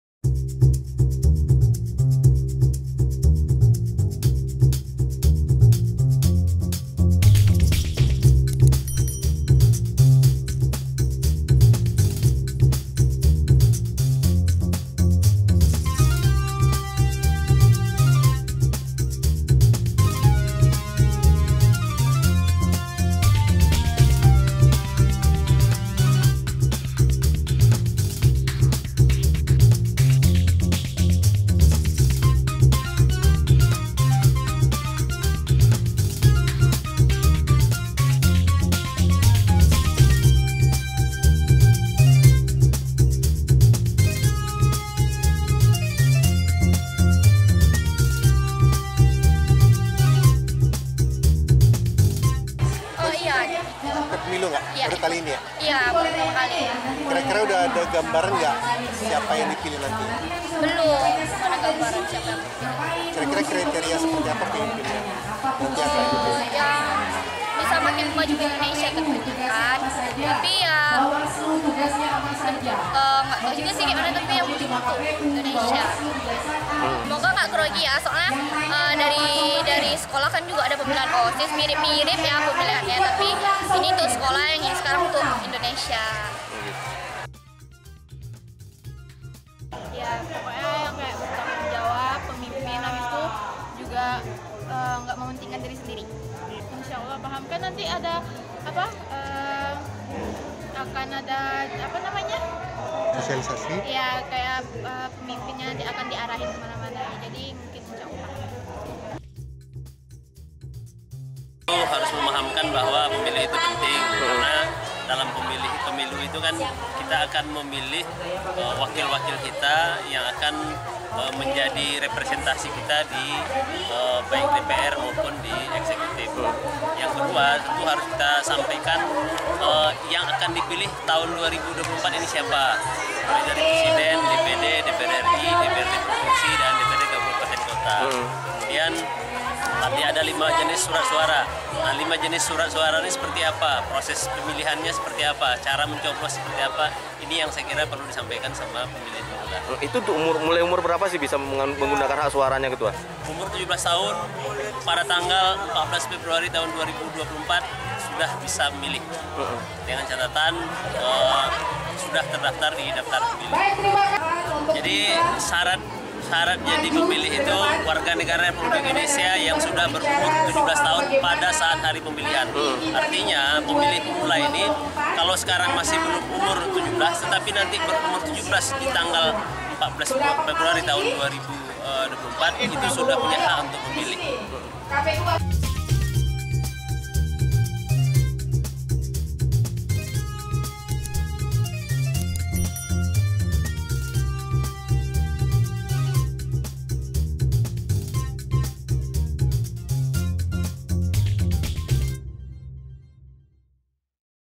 KPU Kota Pekanbaru Gelar Sosialisasi Menggunakan Hak Suara
Jelang Pemilihan Umum pada Februari 2024 mendatang, Komisi Pemilihan Umum (KPU) Kota Pekanbaru gelar sosialisasi menggunakan hak suara saat pemilu.

Sosialisasi digelar di SMA Negeri 5 Pekanbaru, KPU memberikan pemahaman tahapan yang harus dilalui sebelum memasuki Tempat Pemungutan Suara kepada siswa.

Ketua KPU Pekanbaru, Anton Mercianto mengatakan, para calon pemilih pemula harus telah terdaftar di KPU dan berusia 17 tahun di 14 Februari 2024.

Sementara itu, siswa kelas 12, Kalista Adil yang merupakan oemilih pemula mengaku tidak canggung saat nanti menggunakan hak suaranya.

Diharapkan dengan sosialisasi ini, pemilih penula bisa menggunakn hal suaranya dengan baik.

(RiauOnline)